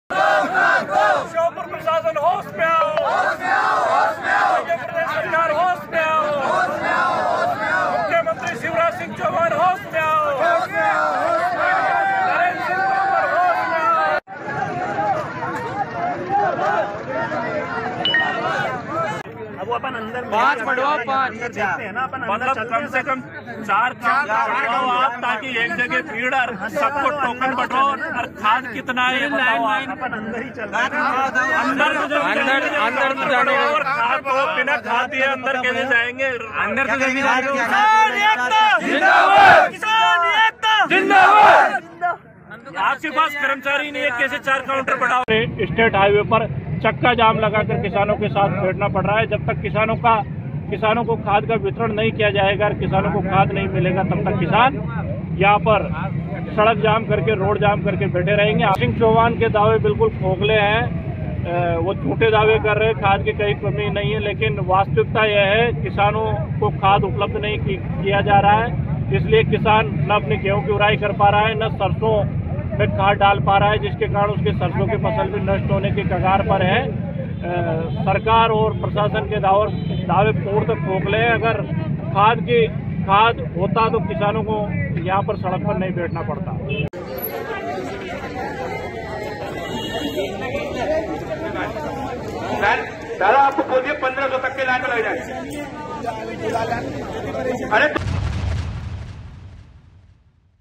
Madhya Pradesh: खाद वितरण केंद्र पर लगी लंबी-लंबी कतारें किसान हो रहे परेशान | Sheopur News
मध्यप्रदेश के श्योपुर में खाद की किल्लत और प्रशासनिक बदइंतजामी के चलते पहले टोकन और फिर खाद लेने लंबी-लंबी कतारों में लगने को मजबूर किसानों का अब धैर्य जवाब देने लगा है यही वजह है कि किसान अब सड़कों पर उतरने लगे हैं.

In Madhya Pradesh's Sheopur, due to shortage of fertilizers and administrative mismanagement, the patience of the farmers, who were forced to stand in long queues to get the token and then the fertilizer, has now started responding, that is why the farmers are now coming out on the streets.